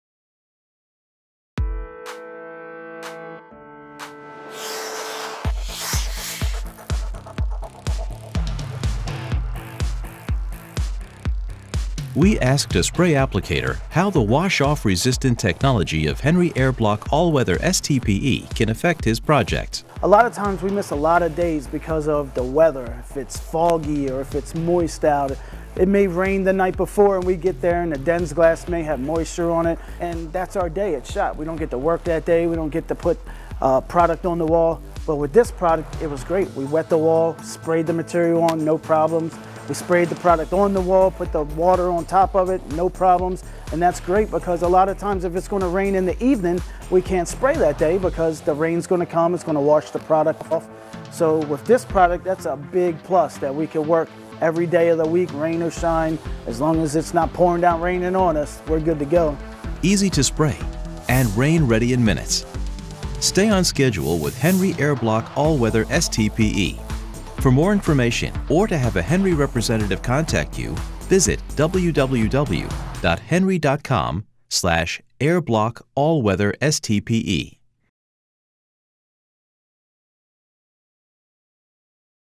Testimonial: Air-Bloc® All Weather STPE™
A vapor permeable membrane with long-term UV resistance, Henry® Air-Bloc® All Weather STPE™ is a single-component, moisture cure Silyl Terminated Polyether (STPE) water-resistive air barrier. Featuring a high solids content and quick cure time, Air-Bloc® All Weather STPE™ can be applied with rain in the forecast, helping to reduce job-site weather delays. Its hydrophobic nature also means it won’t wash off in the rain. Plus, it can be spray-, roller- or brush-applied in temperatures down to 10 °F.  Air-Bloc® All Weather STPE™ is part of the Air-Bloc® portfolio of fluid applied air barriers within the Henry® Building Envelope Systems® family.

 - Rain ready within 30 minutes, helping to reduce job-site weather delays
 - Can be exposed for up to 12 months during construction, providing scheduling flexibility
 - Permanent UV exposure behind open-joint cladding offers design flexibility
 - Part of the fully compatible Air-Bloc® system, including flashings and sealant
 - Vapor permeable membrane allows walls to breathe
 - Sprayable, one-coat application and high coverage rate for a faster, quicker installation